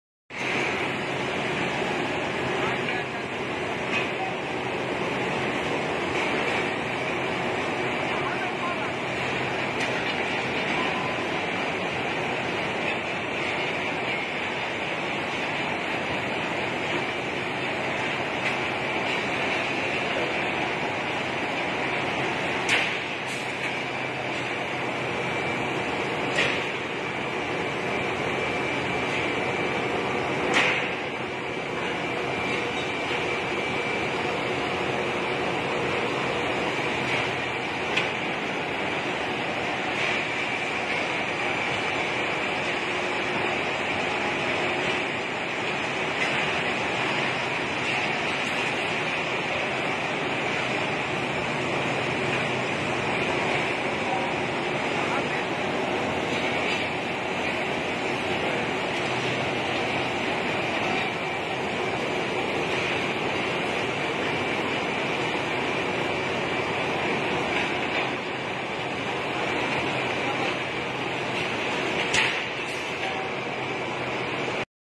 Steel Rolling Mill - Hot Rolling TMT Bars
This is the exclusive video about  Steel bar Manufacturing processing Line

The video shows the Steel Rolling and Manufacturing Process of Steel Bars in Pakistan Rolling Mills. Making of 60 G and 40G Steel Bar